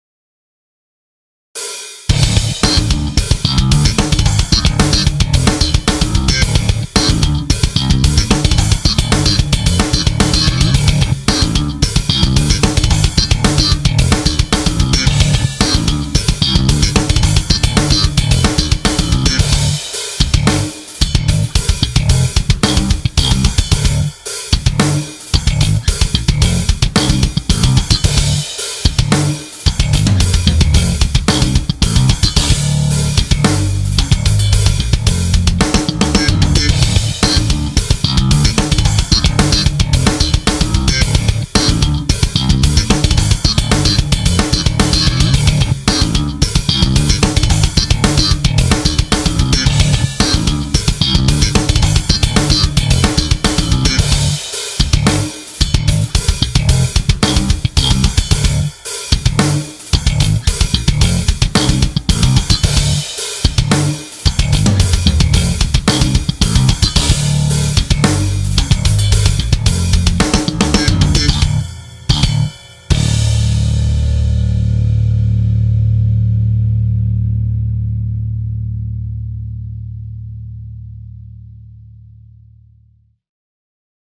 Ernie Ball, Music Man Stingray 4 HH (Nick Lombardi)
Sparatic Rhythms while Slapping the bass to a crazy drum beat!